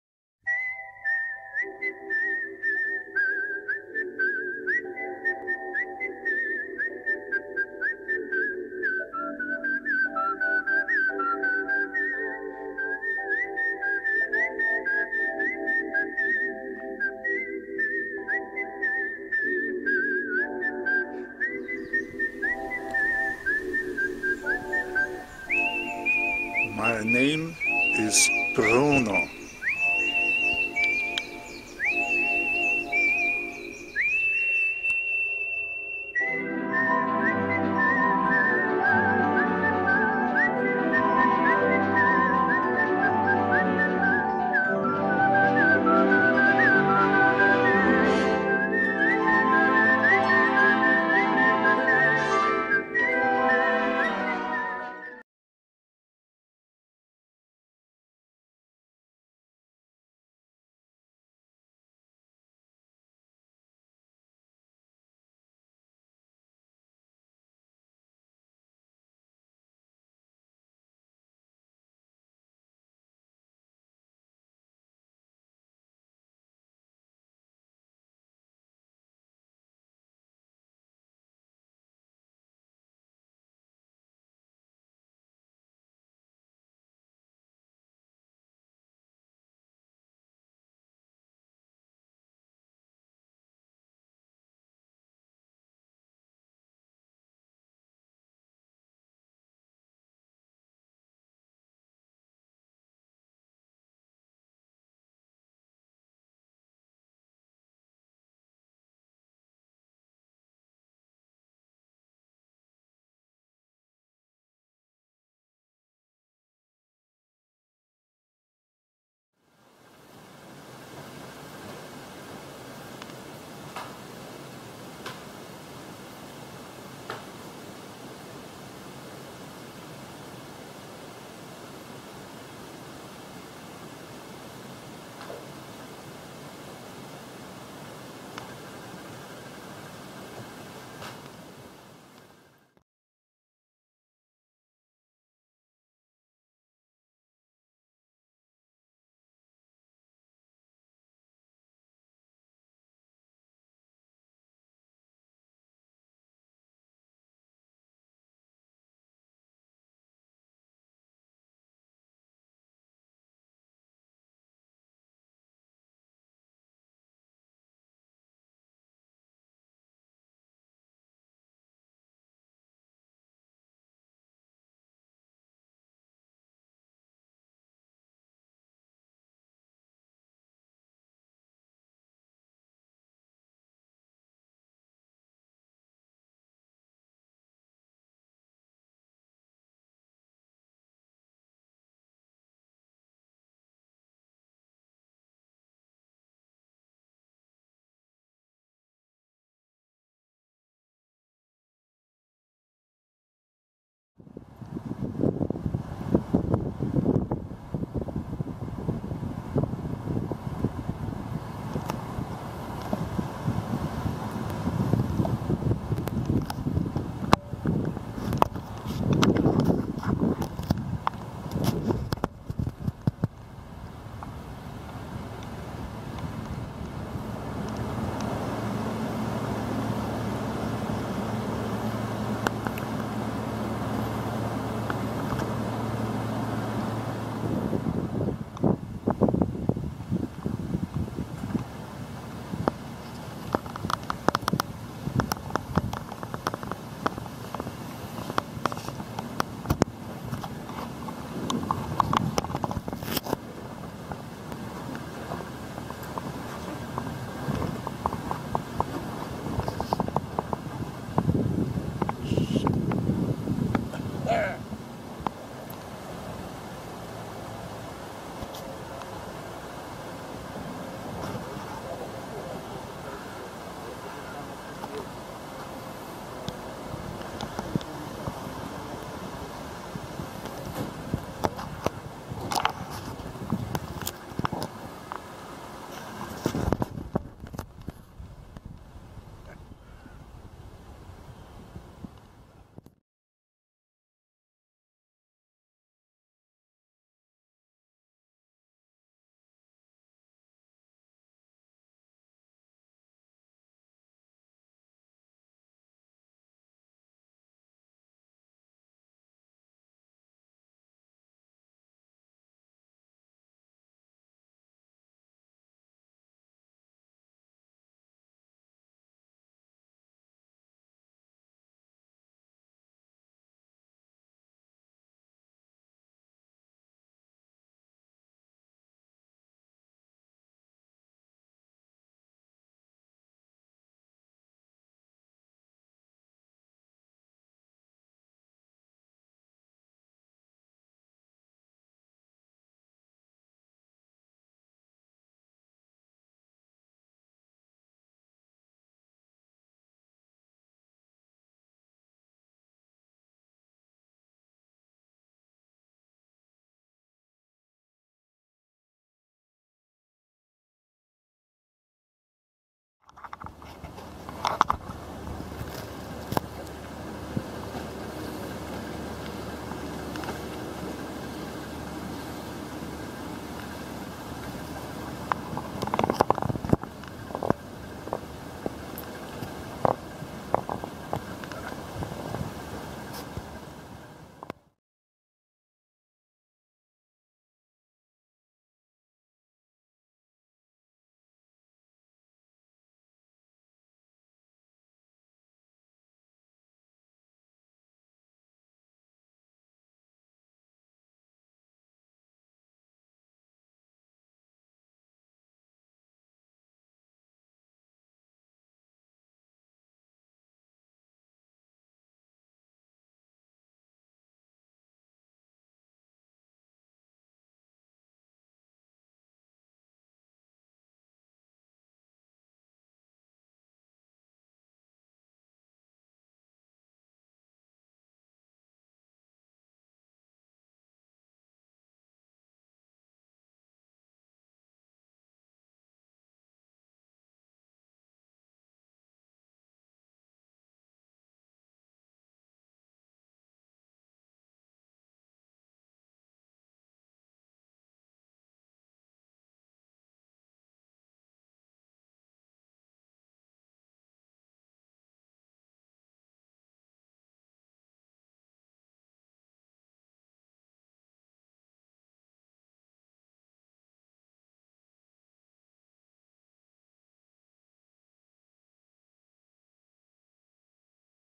2019 07 30 & 2019 08 12 Sir Bruno's daily Stroll to DISTRICT COURT & BLUE DANUBE 00 07 51
Sir Bruno had to visit the District Court to get some Information, and took pics of the exhibited Paintings, which followed on another Day by some long Stroll on the Shores of the Blue Danube.